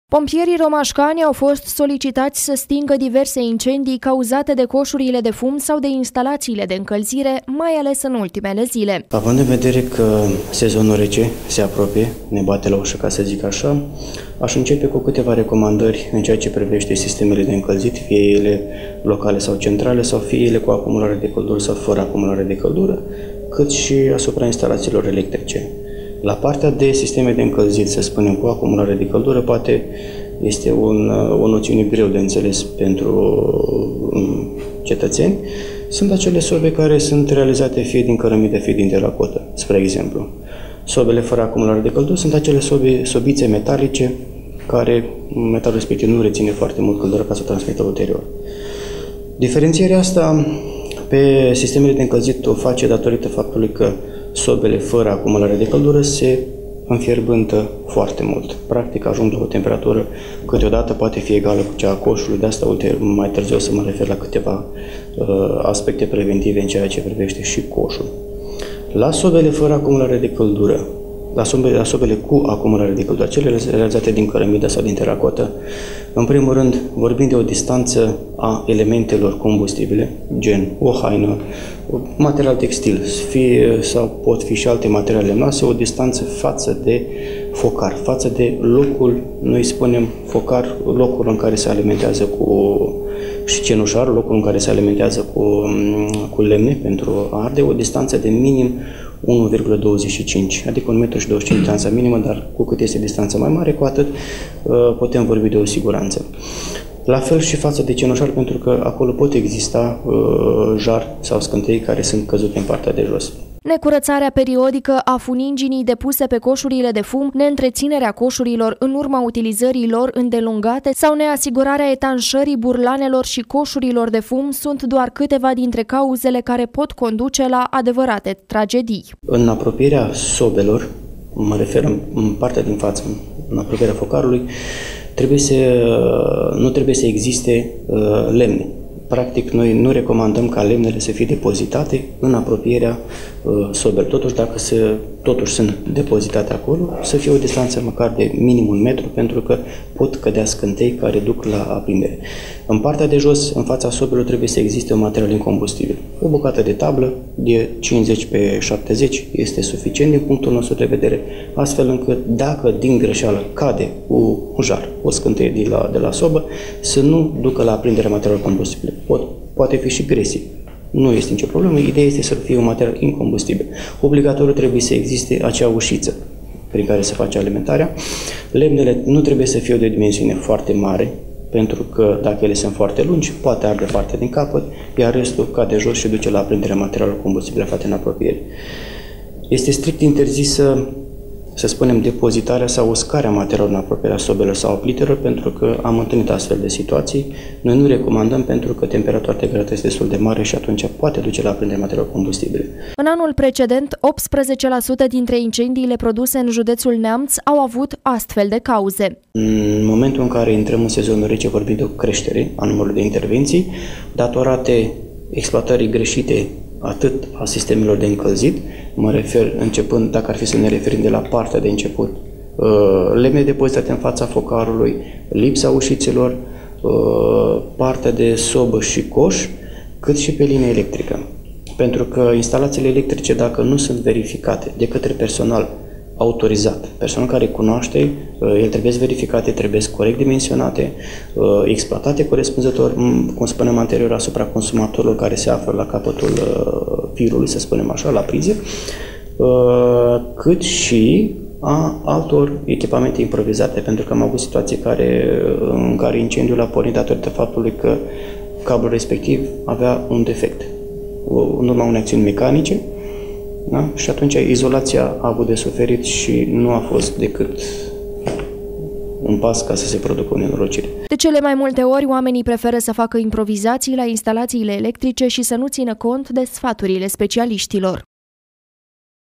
Improvizațiile și coșurile de fum te pot lăsa fără casă
Pompierii romașcani au fost solicitați să stingă diverse incendii cauzate de coșurile de fum sau de instalațiile de încălzire, mai ales în ultimele zile.